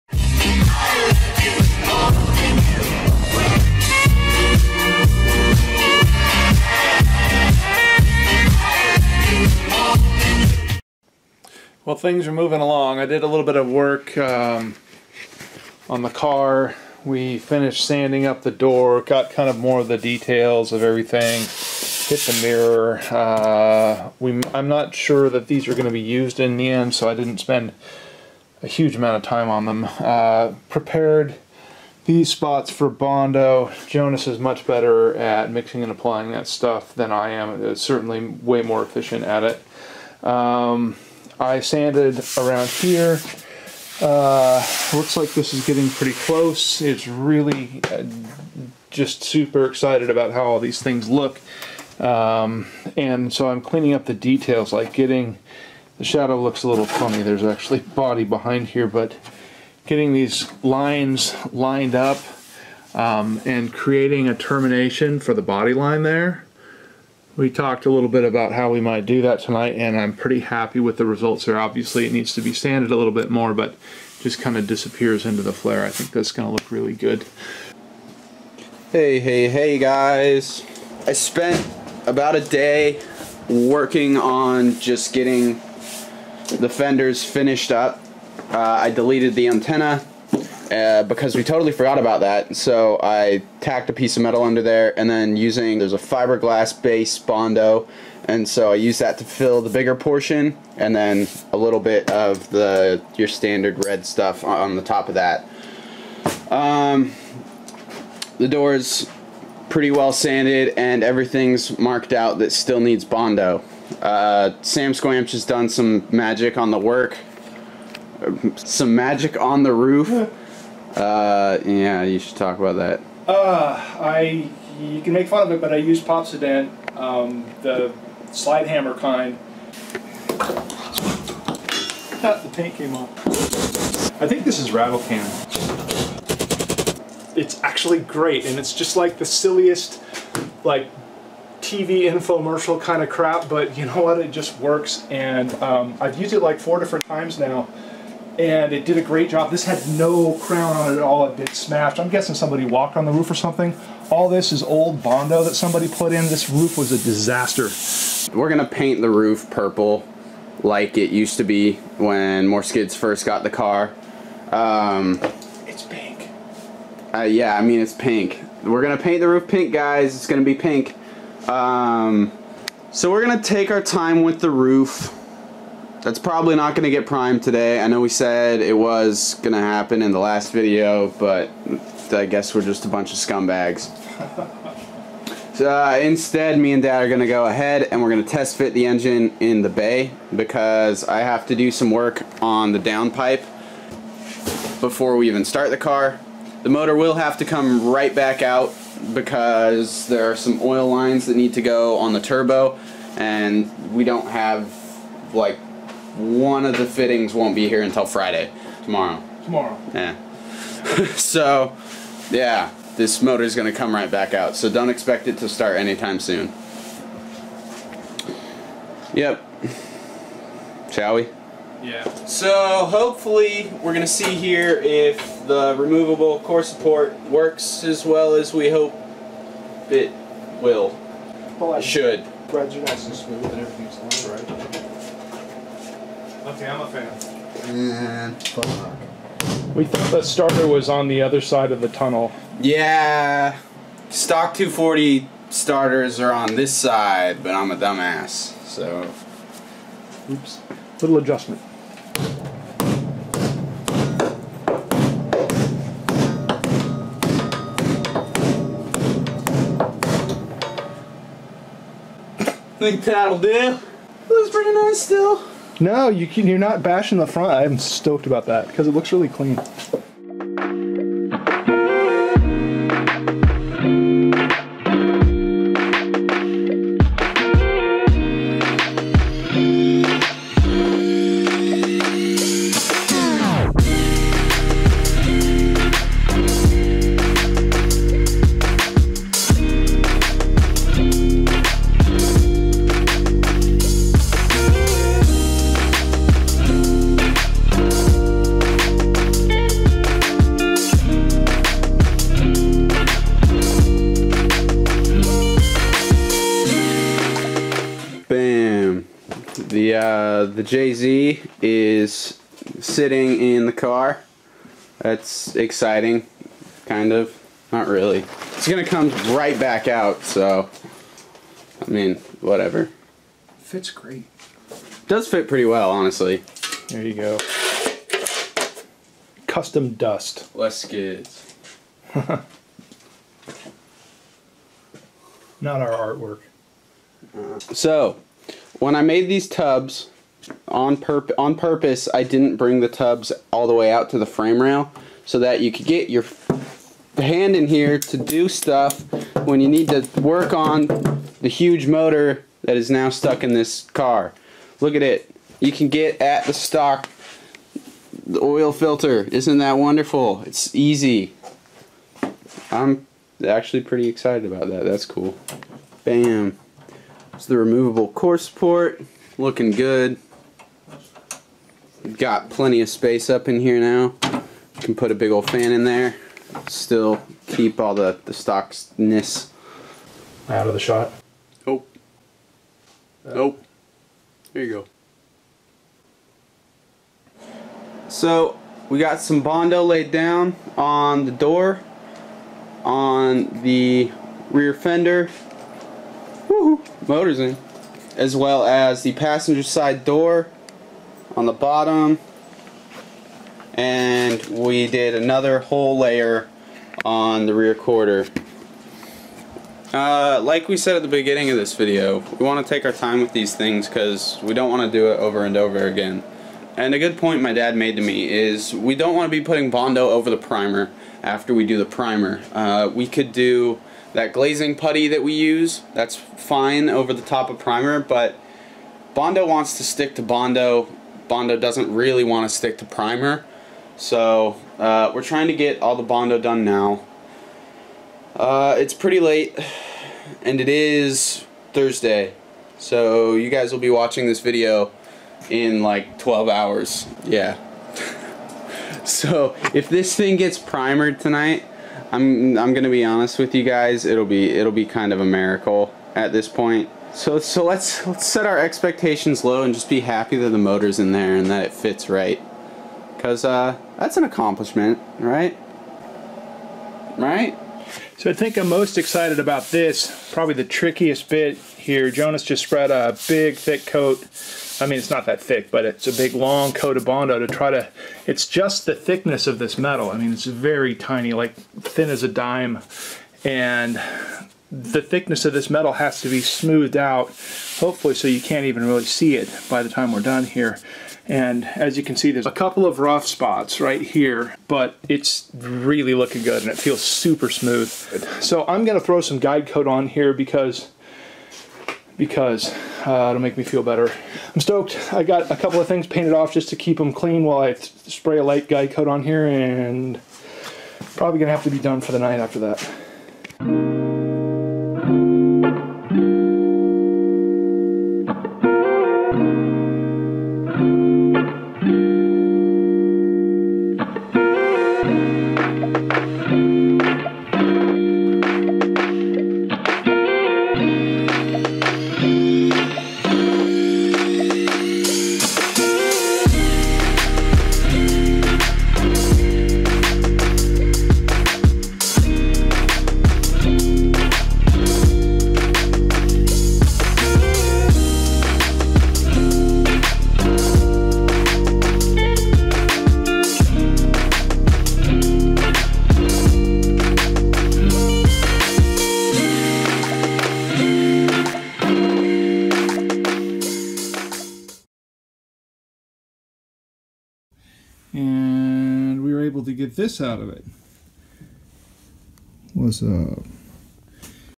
S13 Daily Skid Rebuild | TEST FITTING the 1JZ in the 240SX + Coating Metal Fenders!
All the boys worked very hard to get the entire car primed, but that goal was a little but too much. We decided to postpone primer until the next video so we have enough time to finish the roof of the car, and finish up the few odds and ends that are left. I'm sorry, but the final product will be much better because of it.

In this video the boys spend a night wrangling the heavy Toyota 1JZ into the freshly painted and tubbed engine bay of the 240sx. The removable core support that Jonas fabricated made toting the engine into the car much easier. It also, made a gape in the engine bay for a set of Mishimoto fans and a shroud! After the motor was in we worked together to slap bondo on the remaining dents and dings of the car... Hours later Samsquanch appeared and coated the fenders in guide coat so everyone could get a good look at the metal widebody!